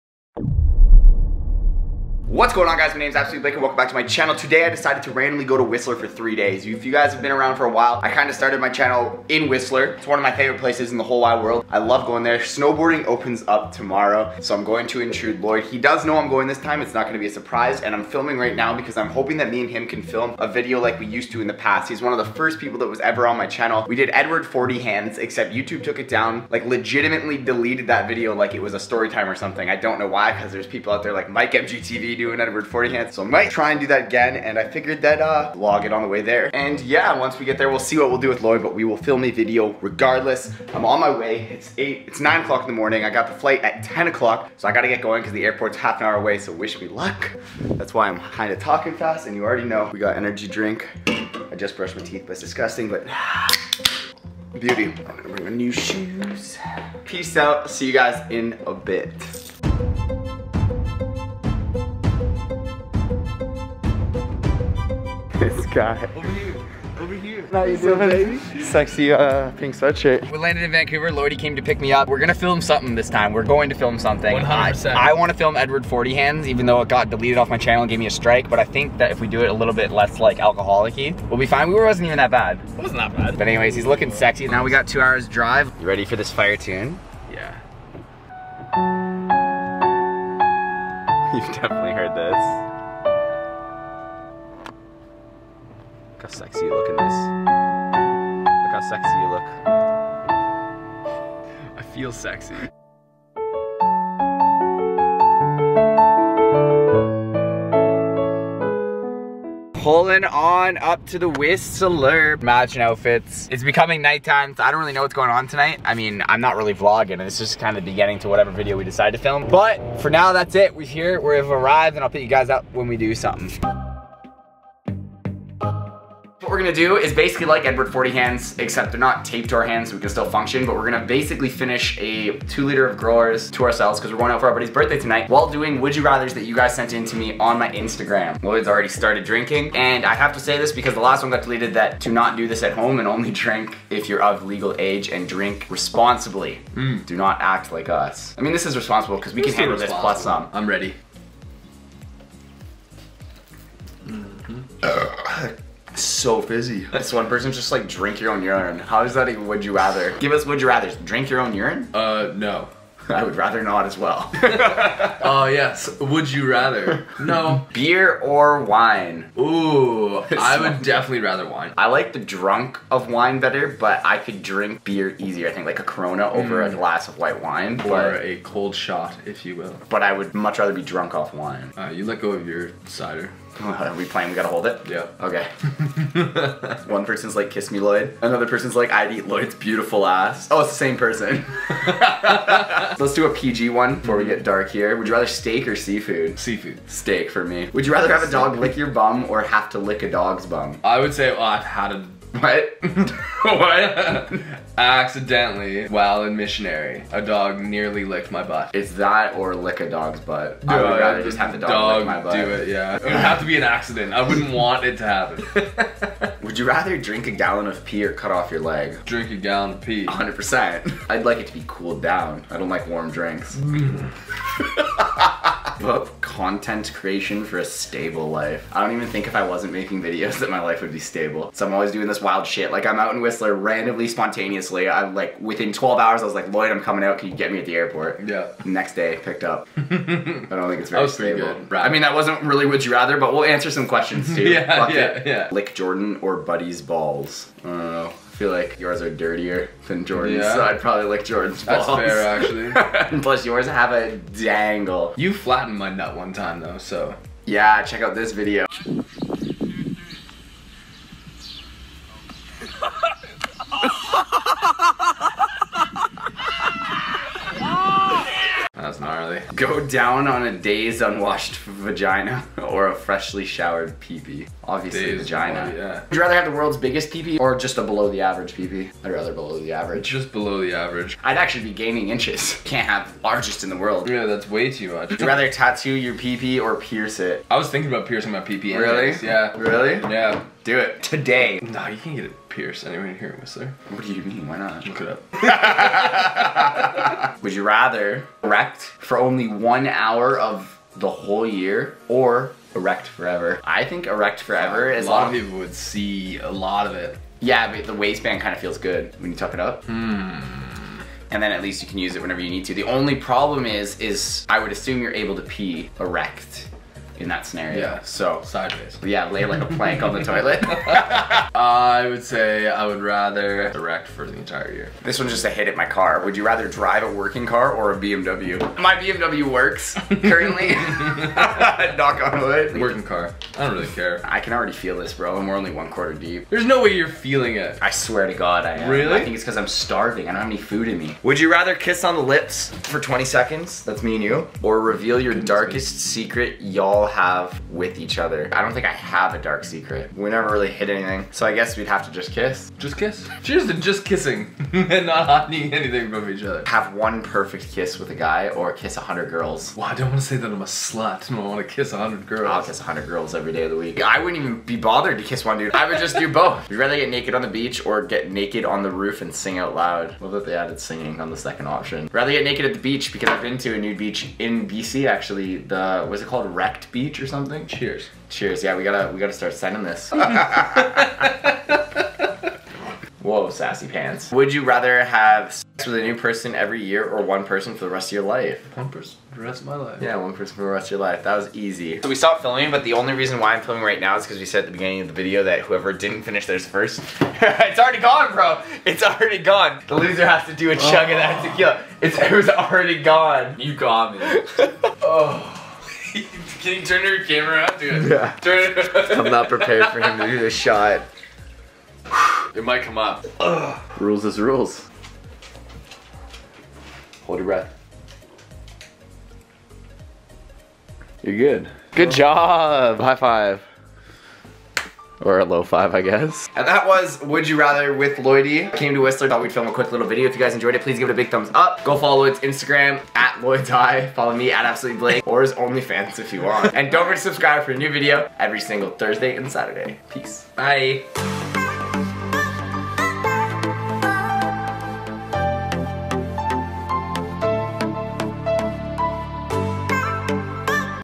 KISS OR REVEAL OUR DARKEST SECRET | Would You Rather W/ Lloyd
Playing would you rather with my longest best friend Lloyd. Watch to find out if we would rather kiss or reveal our darkest secret!

Personal Cameo Shoutout?